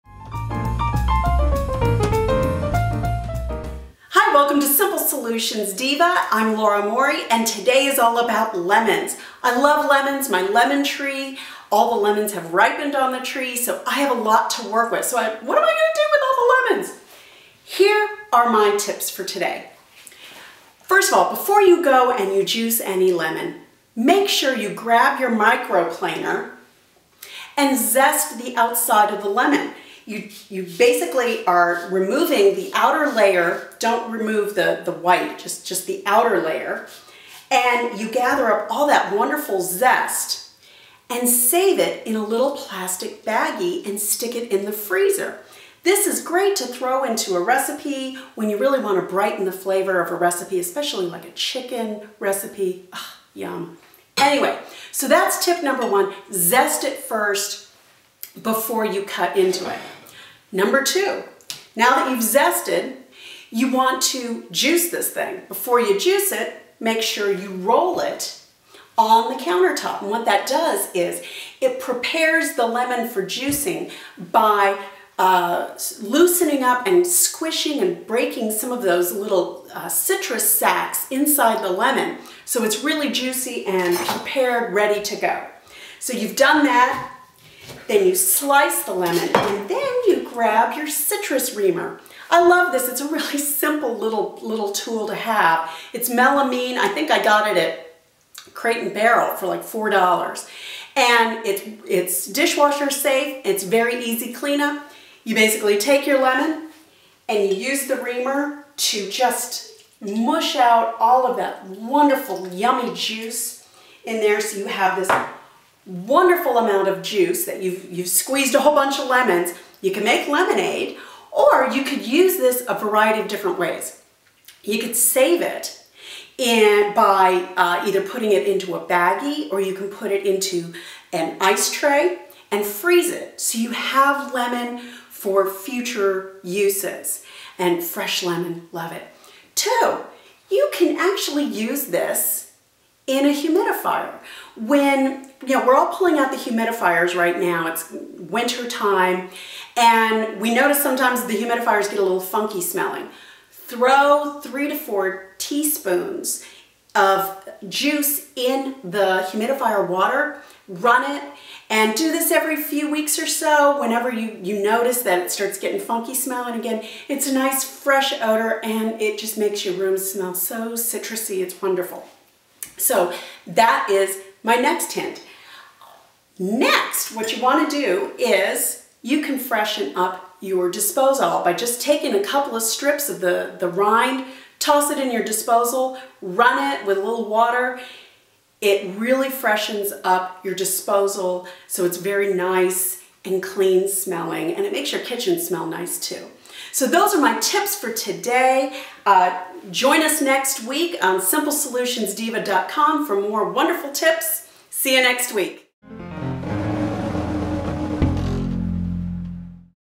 SimpleSolutionsDiva.com - What To Do With Lemons!
Laura Morey, your Simple Solutions Diva, gives tips on how to use lemons.